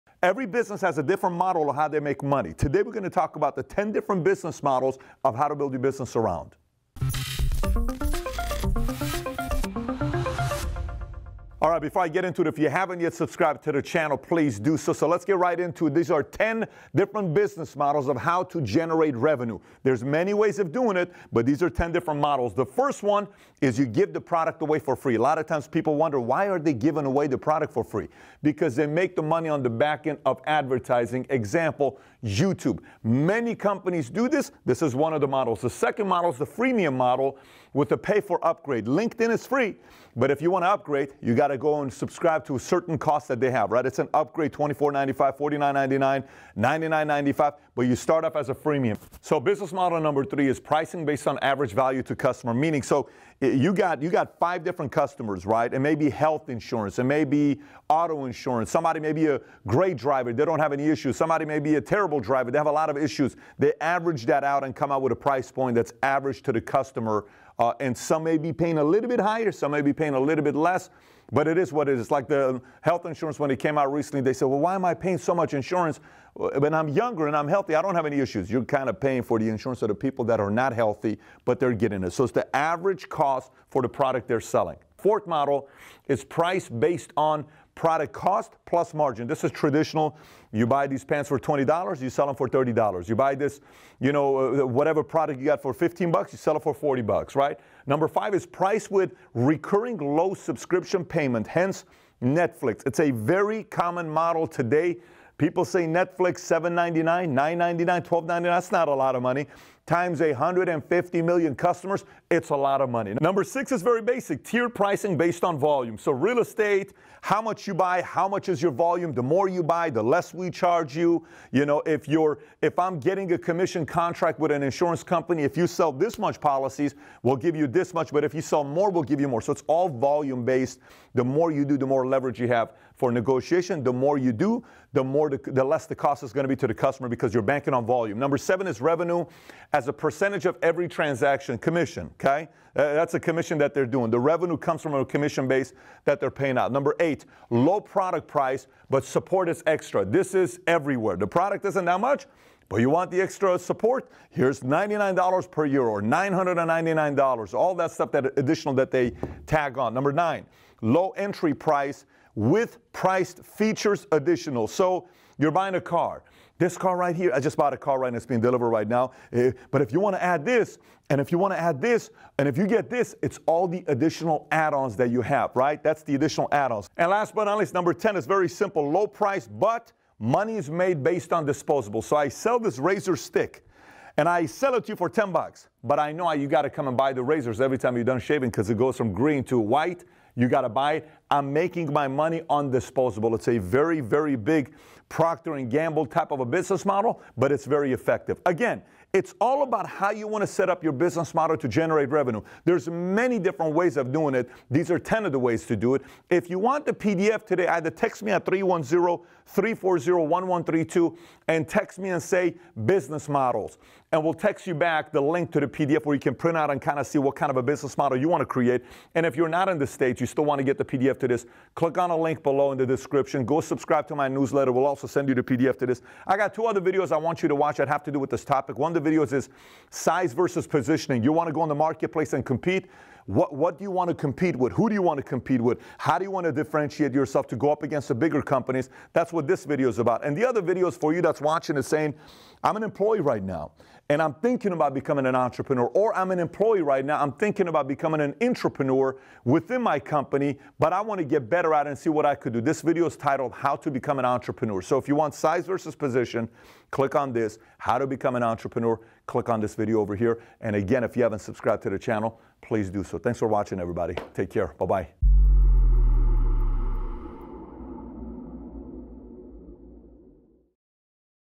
10 Business Models for Every Entrepreneur
Check out the new home for all things Valuetainment!

Ever wanted to get personally mentored by Patrick Bet-David, but can't make the trip? Check out his 2021 Virtual Business Planning Workshop!

In this video Patrick Bet-David talks about 10 different business models and how they generate revenue.